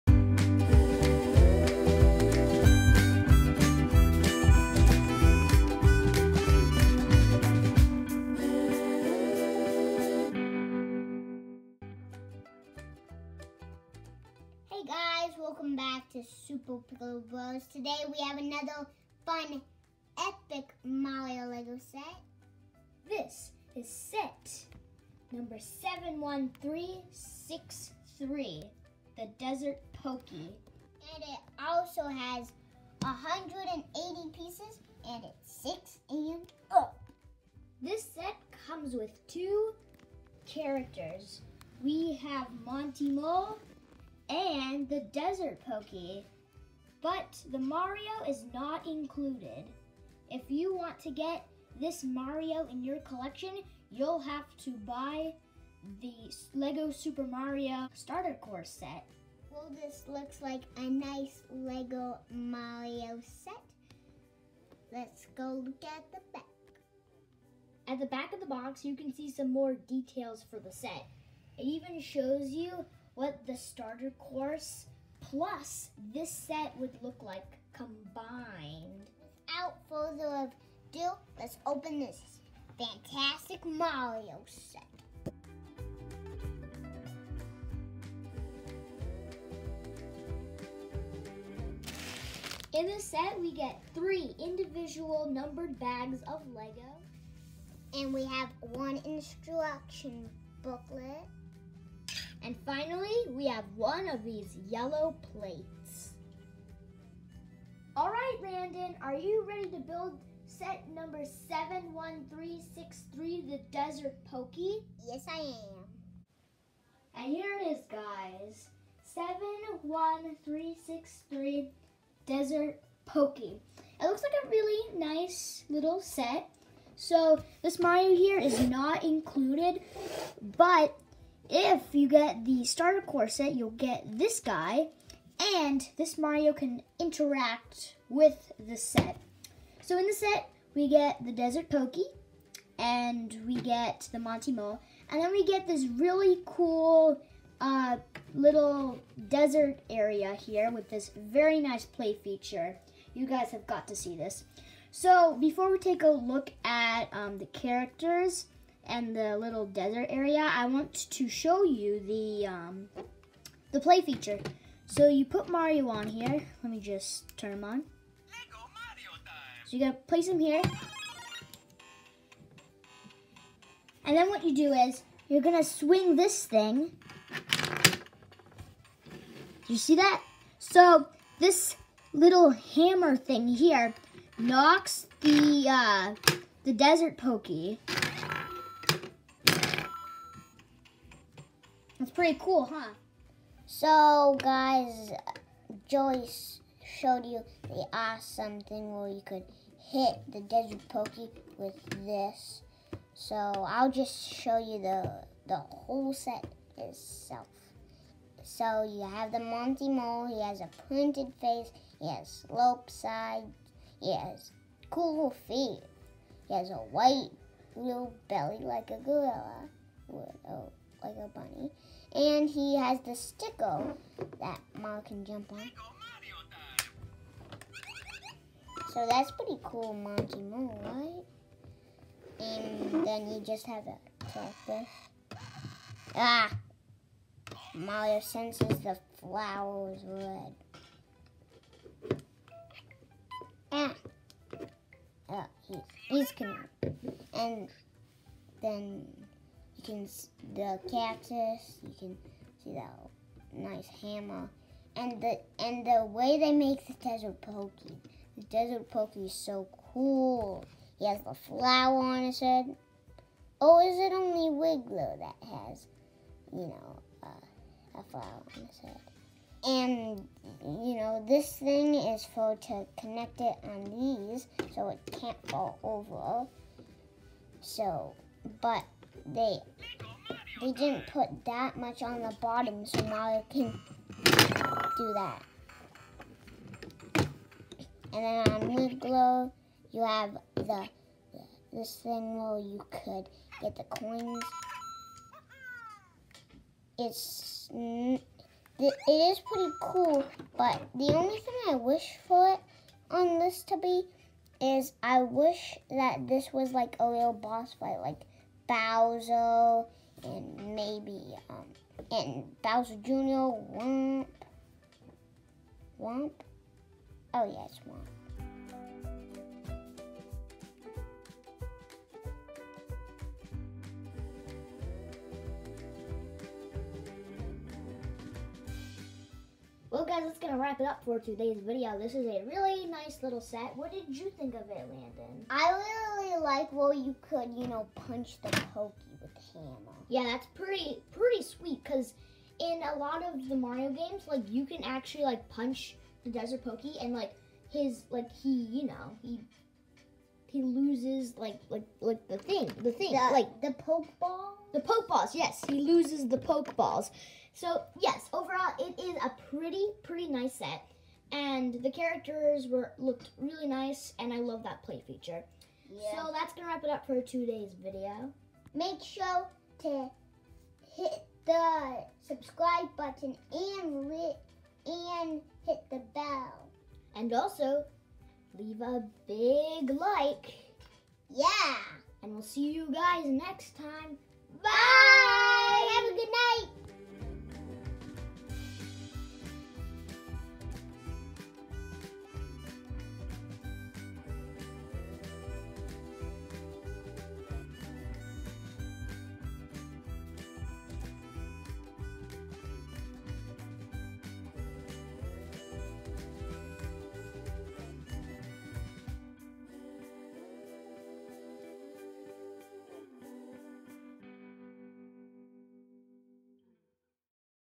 LEGO Super Mario 71363 Desert Pokey Unboxing and Reviewing with my brother!
00:00 Intro
01:19 Unboxing
01:56 Aftermath
02:44 Desert Pokey Play Feature
03:13 Monty Mole
03:52 Desert Pokey
05:18 Conclusion